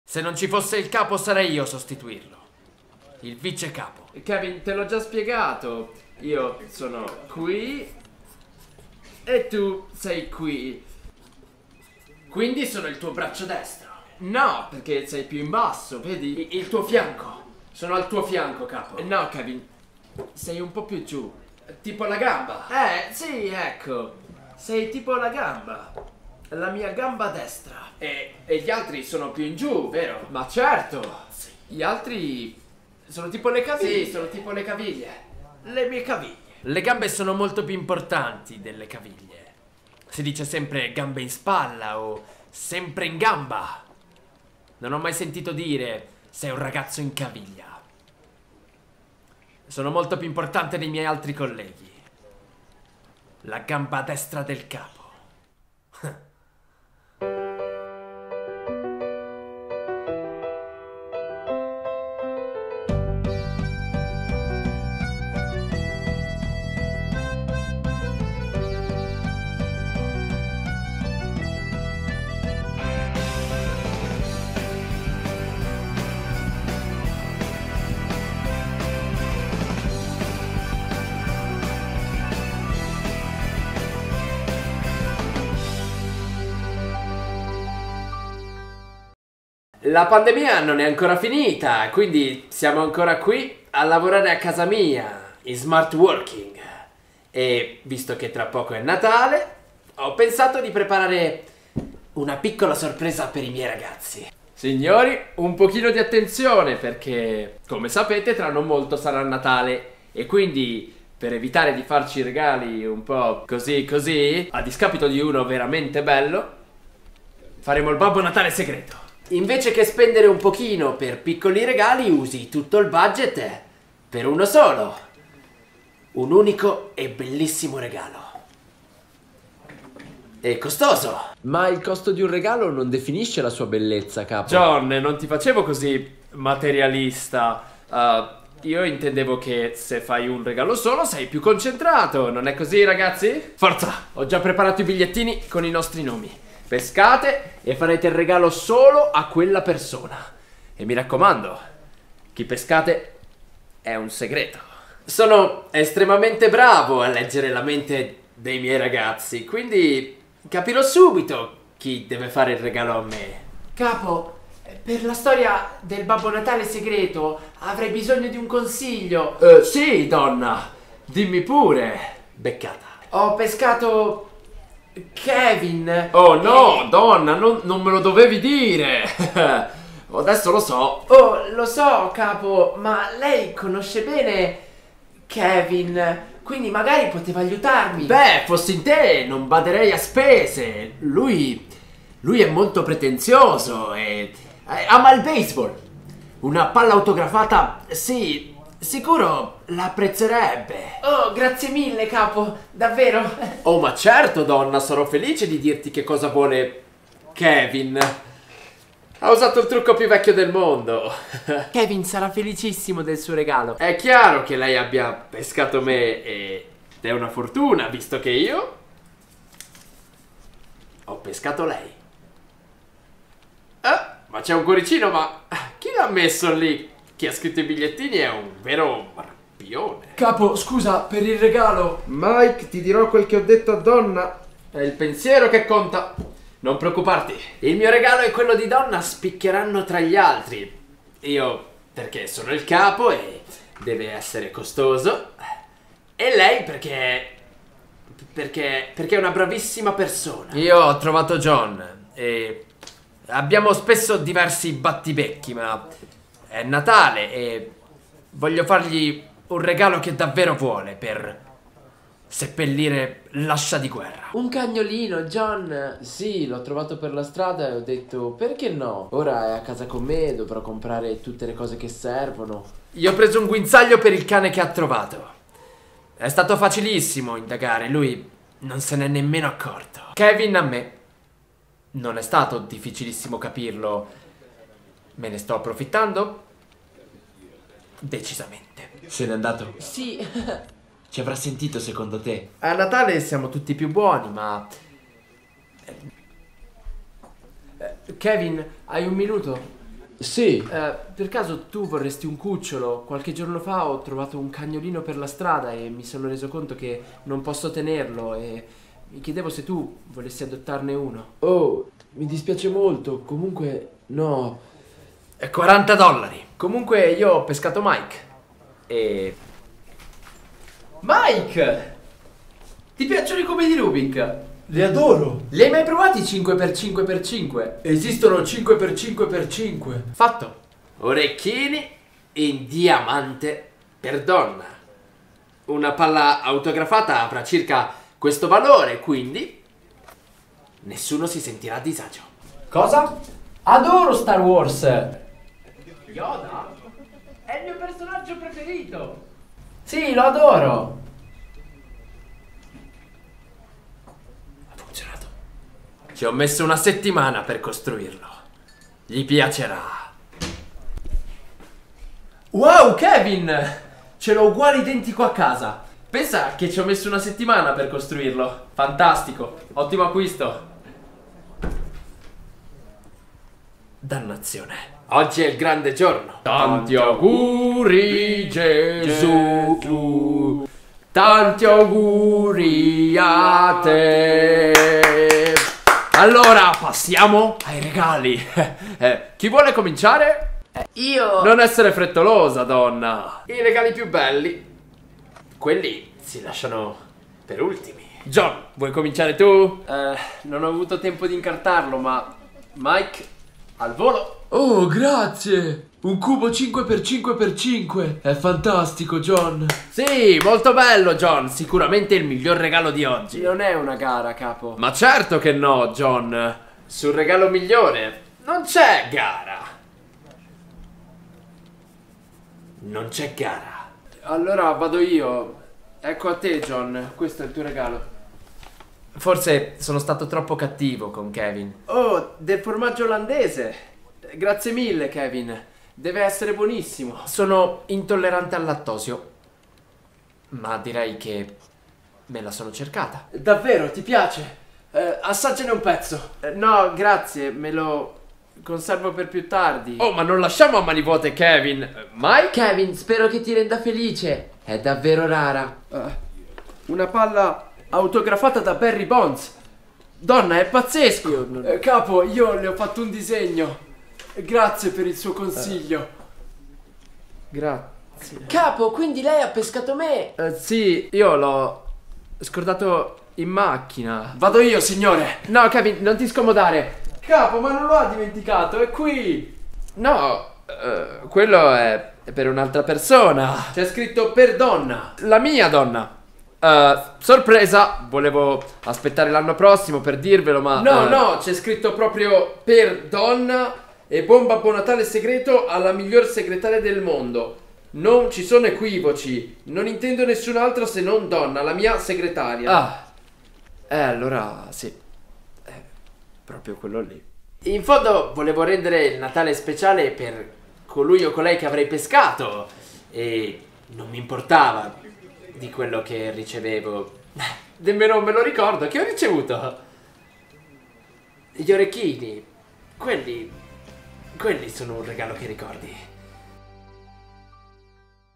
The Office a Natale interpretato da @Luca__Sironi | Secondo episodio
Eccoci al secondo episodio di The Office interpretato dal mitico Luca Sironi. In questa puntata ecco un piccolo spoiler: il secret Santa! Non possiamo dirvi altro, cliccate play 😁